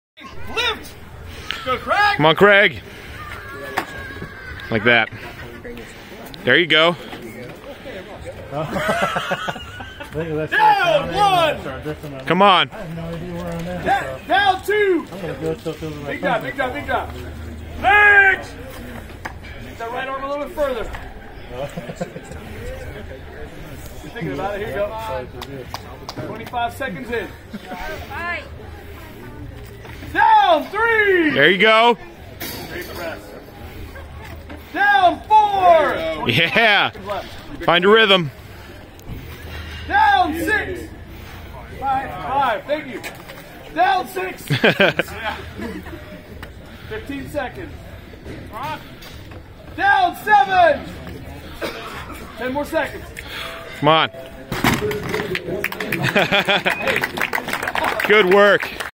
Overhead Motorcycle Press
I got to fulfill a childhood dream by doing overhead press with a motorcycle.